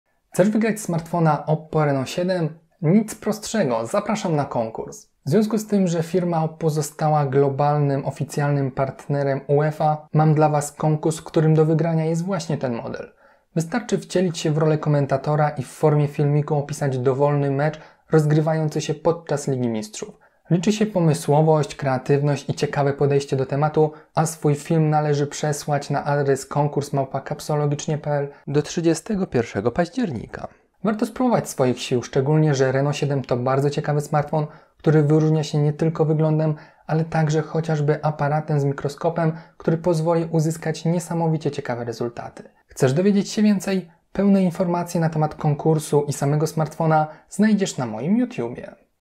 Wygraj OPPO Reno7! 🥳📲 KONKURS OPPO x UCL
Film powstał na zlecenie marki OPPO, która jest fundatorem nagrody w konkursie. Nagrodą jest smartfon OPPO Reno7.

Główne zasady konkursu:

1) Polub ten film.
2) Zaobserwuj mój profil na YouTube Kapsologicznie (Kacper Żarski).
3) Skomentuj w formie filmu swój ulubiony mecz i prześlij swoją odpowiedź na adres Postaw się w roli komentatora i opisz wybraną rozgrywkę.
4) Najbardziej kreatywna odpowiedź zostanie nagrodzona smartfonem OPPO Reno7.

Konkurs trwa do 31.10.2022

Powodzenia! 😊